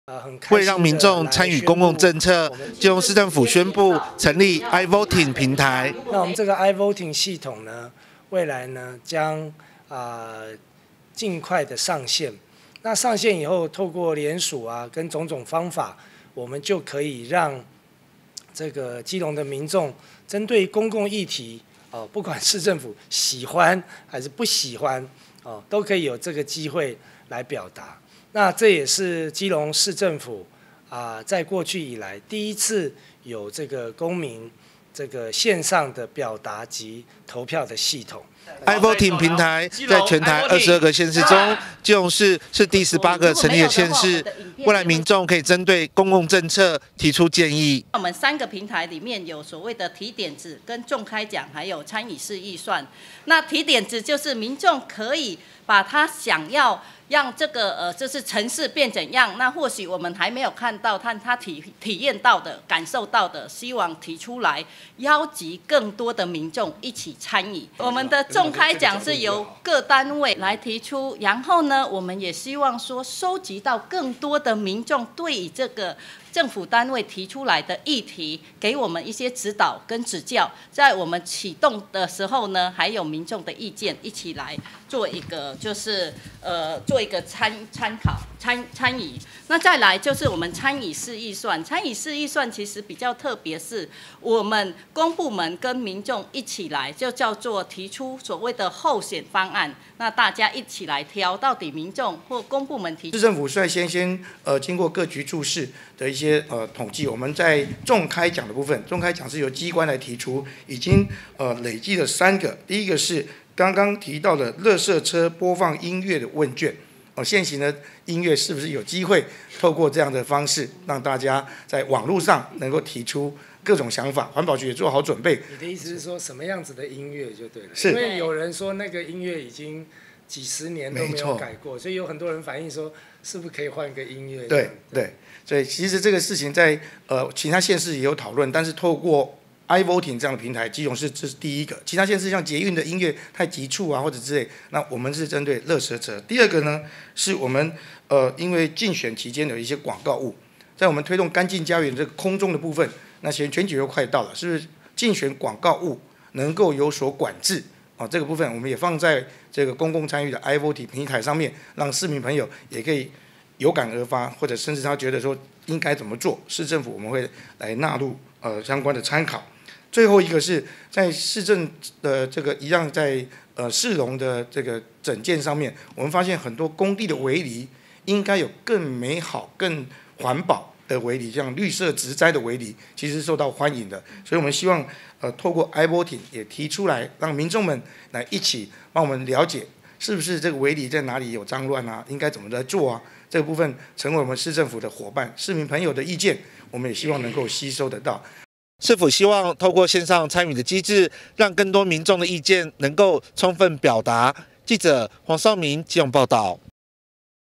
i-Voting｜謝國樑｜基隆市府成立公共政策網路平台 22日起上線
(中嘉吉隆新聞 記者黃紹銘/基隆報導) 基隆市政府宣布成立「i-Voting公共政策網路參與平台成立」，22日開放上線，民眾可以針對公共議題提點子、參與討論和投票。市長謝國樑說，實體公投需在規定時間點內進行，但透過基隆i-Voting則能隨時表達民意。

為讓民眾參與公共政策，基隆市政府宣布成立「i-Voting」平台

「i-Voting」平台，在全台22縣市中，基隆是18個成立的縣市，未來民眾可針對公共政策提出意見。

市府目前擬定3項提案，別為「垃圾車播放音樂問卷」、「實施競選廣告物減量問卷」、「工地圍籬綠美化推動問卷」，市府希望透過線上參與機制，讓更多民眾意見能充分表達。